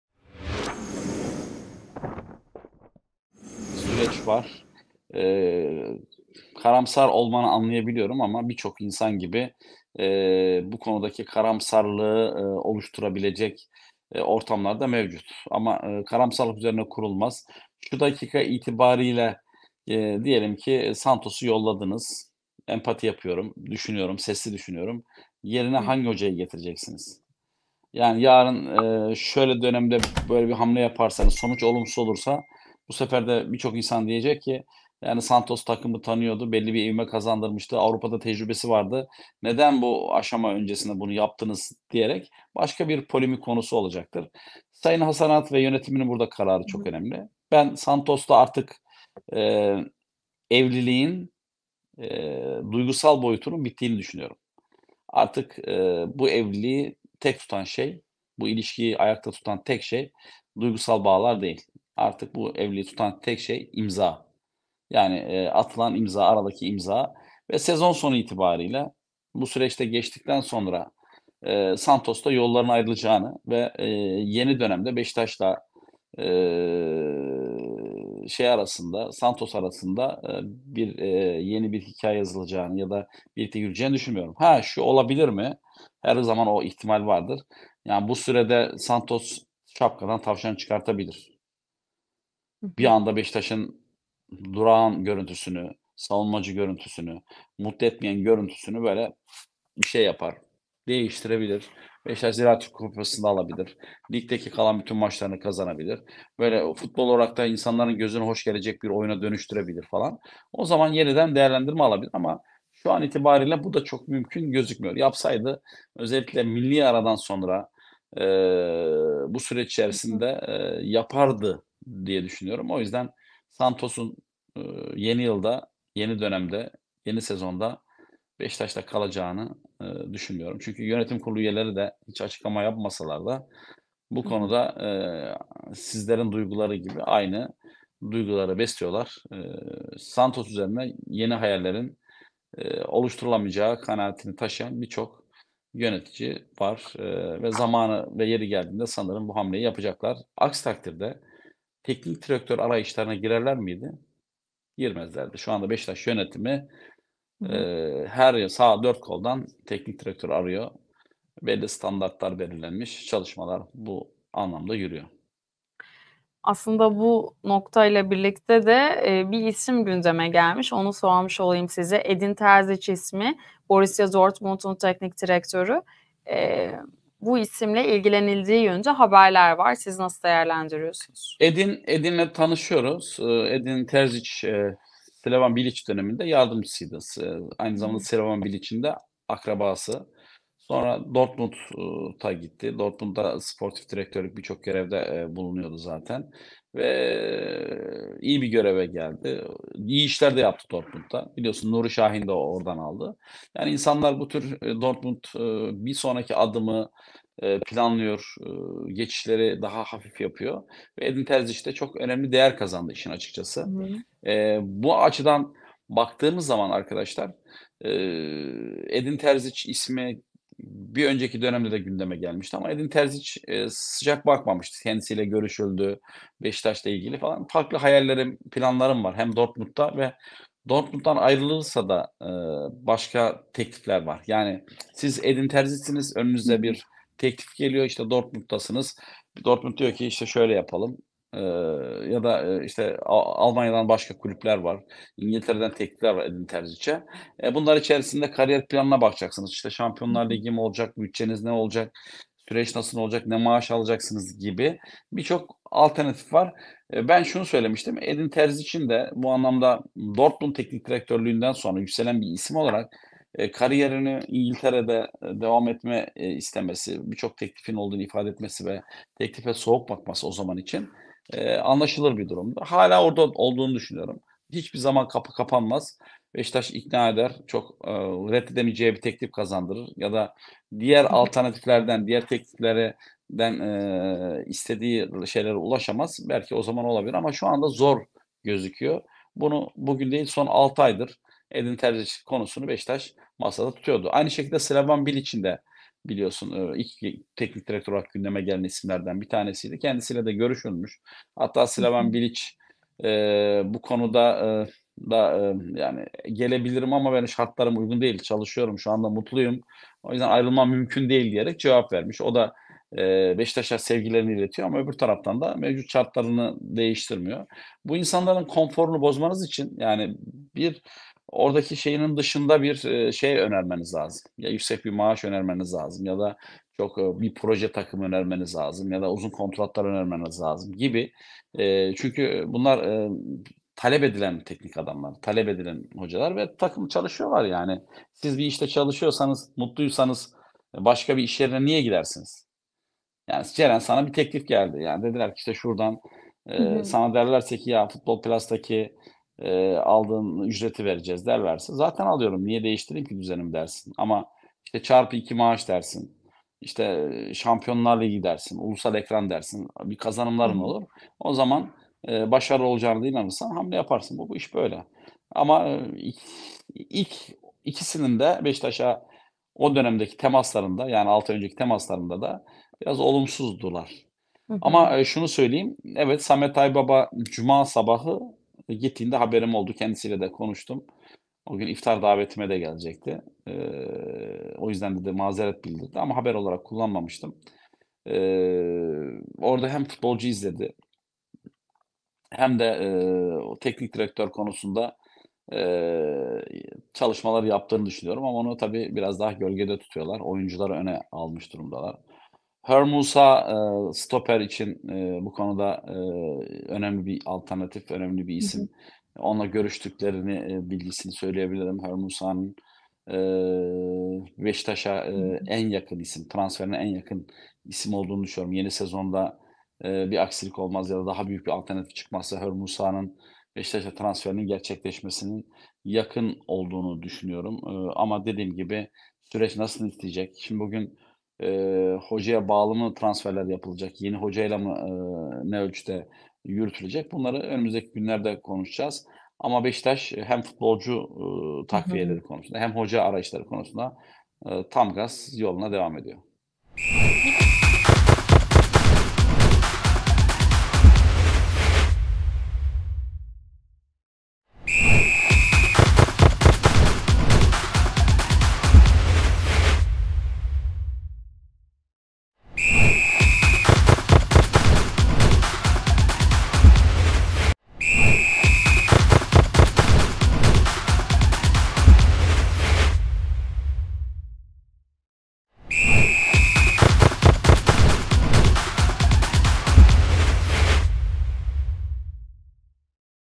Beşiktaş'ta Santos yerine Edin Terzic! | Fatih Doğan & Ceren Dalgıç
Fatih Doğan, siyah-beyazlı ekipte yaşanan son gelişmeleri Futbol Plus ekranlarında Ceren Dalgıç'a aktarıyor.

Beşiktaş yönetimi, son maçlarda alınan kötü sonuçlar ve taraftarların tepkisi nedeniyle Fernando Santos'un yeni sezonda devam etmesini beklemiyor.

Yönetimin göreve getirmeyi düşündüğü hoca adayı şu anda Borussia Dortmund'u çalıştıran Edin Terzic olduğu belirtiliyor. Ancak, mevcut görevi nedeniyle Terzic ile henüz temas kurulmadığı ve sezon sonunda görüşmelerin yapılacağı ifade ediliyor.

Beşiktaş Başkanı Ahmet Nur Çebi'nin yakın çevresine, "Şu anda görevde olduğu için Terzic ile temas kurulmasını doğru bulmuyorum. Sezon bitince oturur konuşuruz" dediği öğrenildi. Çebi'nin, "Yeni sezonda takımın başında onu görmeyi hayal ediyorum" şeklinde ifadeler kullandığı belirtiliyor.

Bu sezon Borussia Dortmund'un başında 39 maça çıkan 41 yaşındaki Alman çalıştırıcı 21 galibiyet, 11 beraberlik ve 5 yenilgi aldı. Terzic'in maç başına ortalama 1.90 puan aldığı görülüyor.